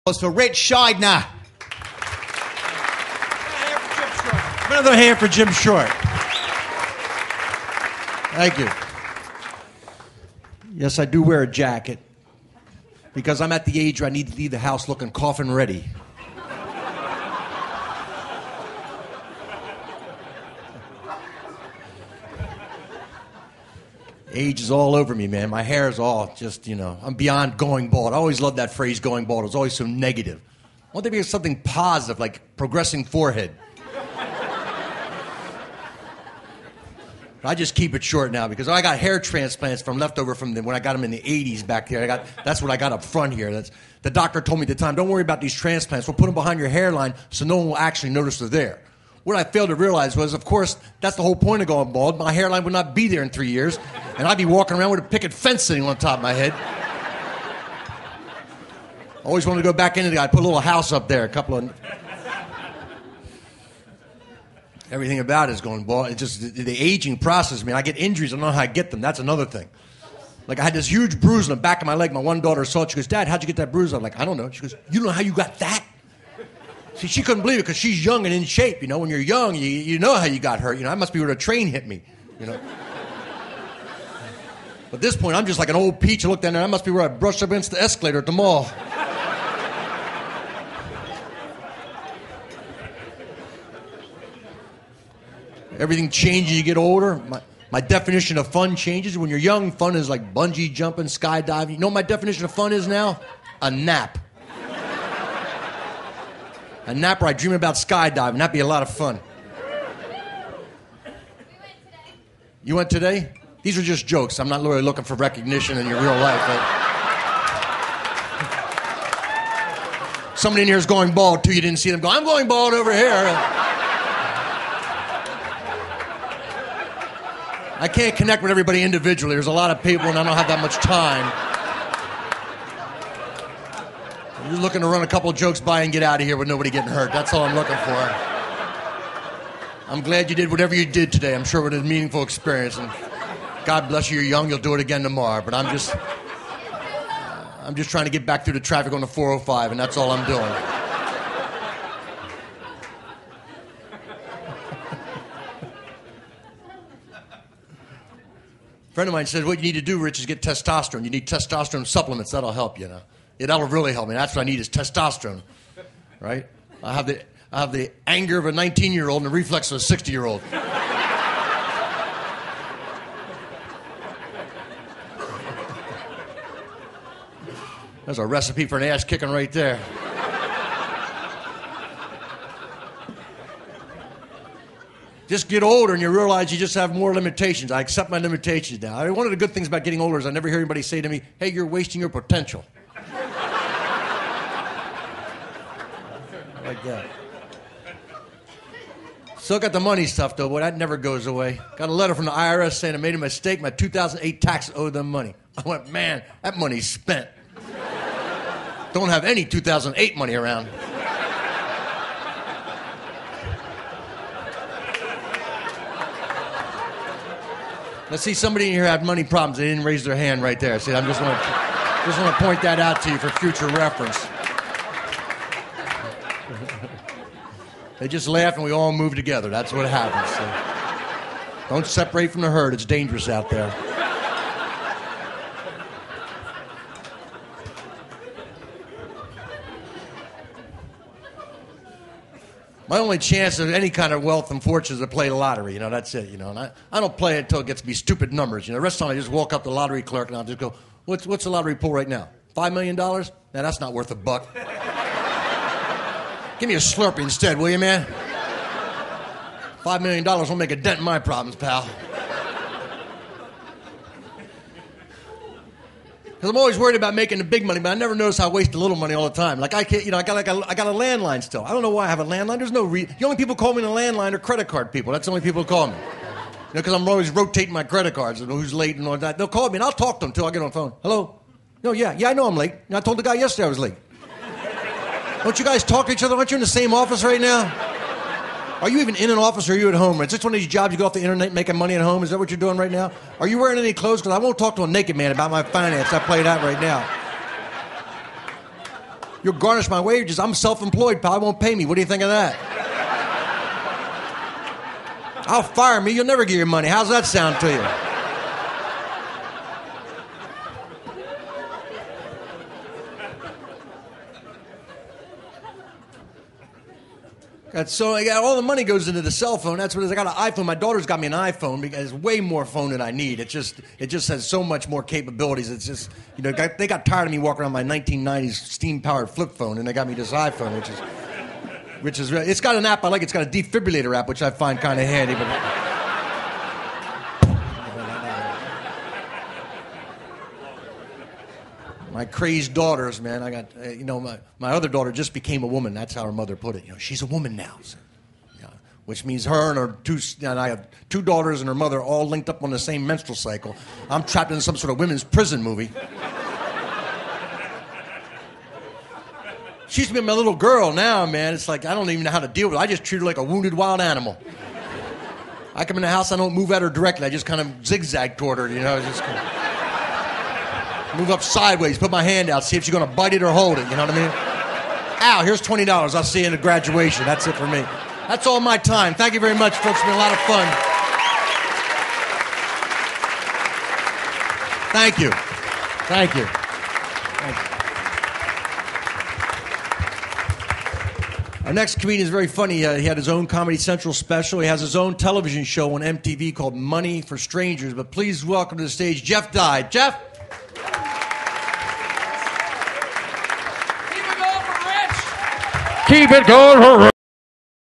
Ritch Shydner - Comedy & Magic Club
Performing at the Comedy & Magic Club in Hermosa Beach, Ca. June 12, 2012